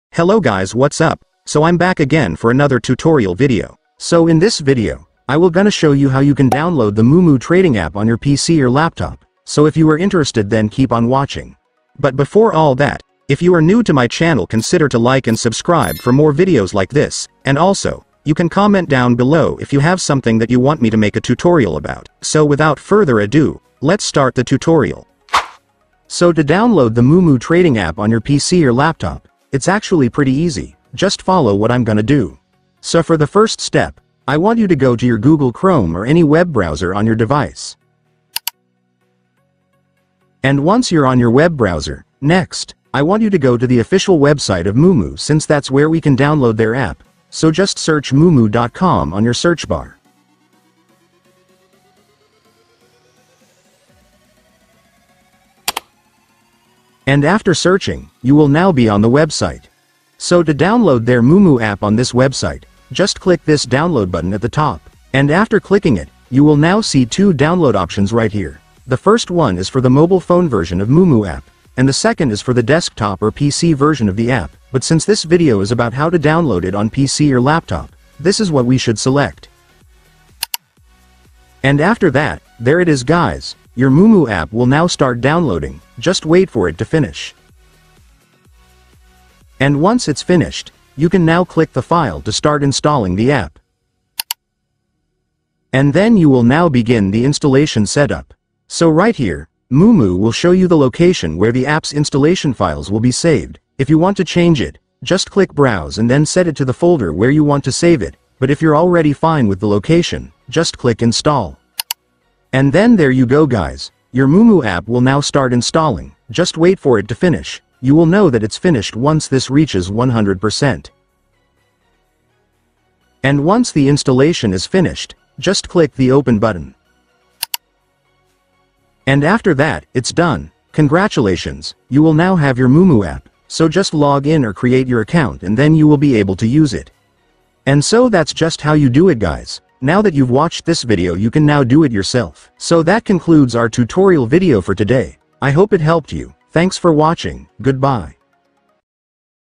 How to Download Moomoo App on PC or Laptop - Easy Step-by-Step Guide
Learn how to install moomoo trading app in this quick tutorial video.